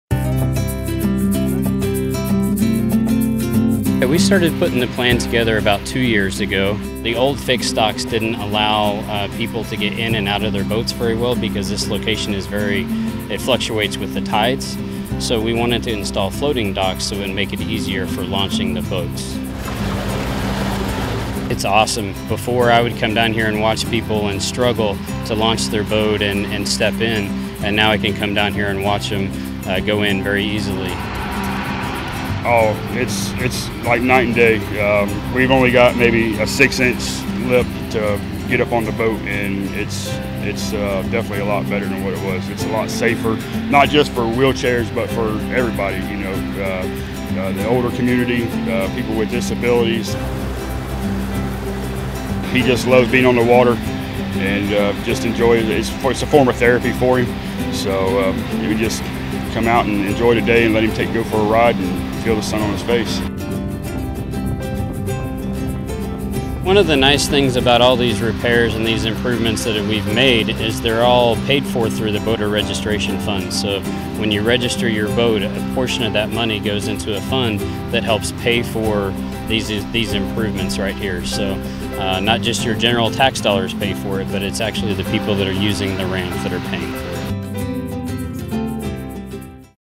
Williams Boat Ramp & Park Reopens After Major Renovations
The Williams Park ramps near Gibsonton re-opened July 1, following extensive upgrades. Trailer-boaters were there, eager to take advantage of the improvements and the ramps' close proximity to Tampa Bay.